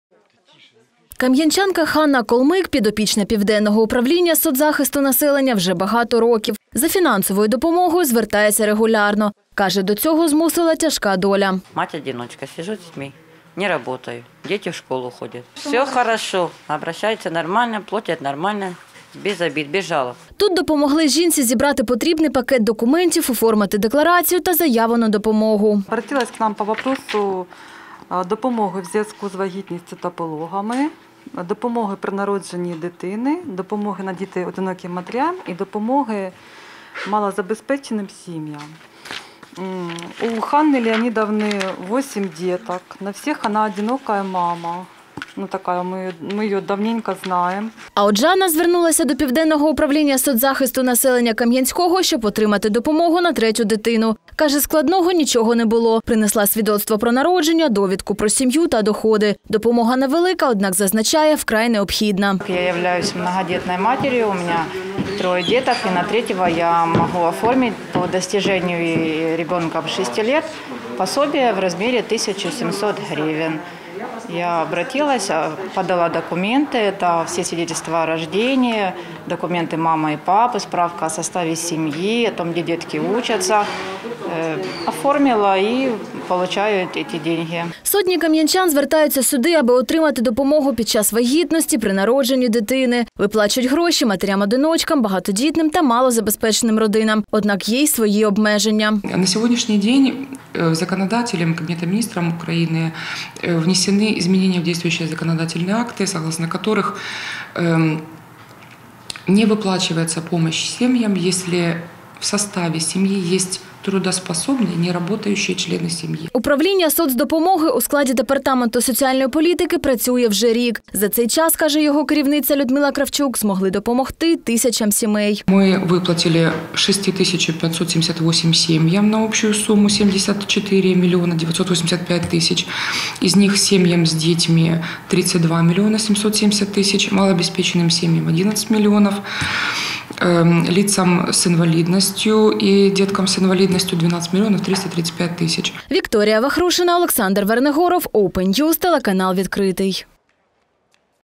Яку грошову допомогу можуть отримати сім’ї з дітьми у Кам'янському
У Кам’янському працює центр соціальної допомоги, де сім’ї з дітьми можуть отримати грошову допомогу. Надається вона  при вагітності, при народженні дитини, за програмою «Пакунок малюка», «Муніципальна няня». Подробиці – далі в сюжеті.